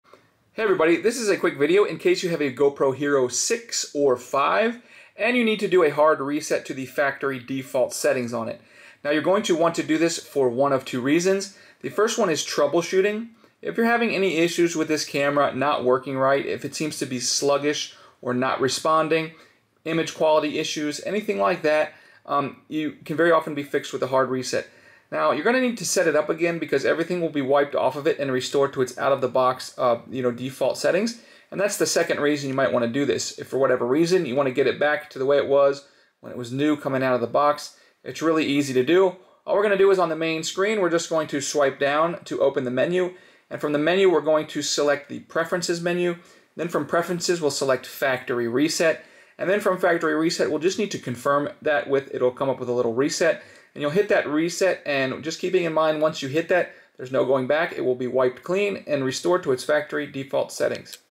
Factory Reset GoPro Hero 6/5
How to troubleshoot or setup starting with a hard reset a GoPro Hero 6 black or GoPro Hero 5  black digital Camera to factory default settings if it is not working or not connecting or if it is frozen or not responding, the factory reset is a good troubleshooting step. You can also restore the factory settings to setup your device as new. Found in the GoPro Hero 6/5 digital Camera user manual.